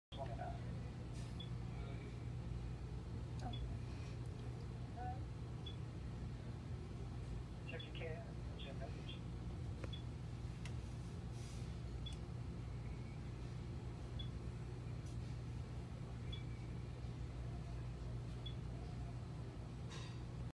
Accused robber crawls through Algiers business - 2
New Orleans police said a suspect shown on surveillance video accused of burglarizing a business on General de Gaulle Drive crawled through the business to avoid setting off an alarm.